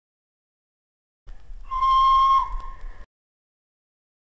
LNER Reid D30 class whistle sound effect
Here is the whistle sound of the LNER Reid D30 class locomotive.

Recorded by Peter Handford.

Photo belongs to Ben Brooksbank.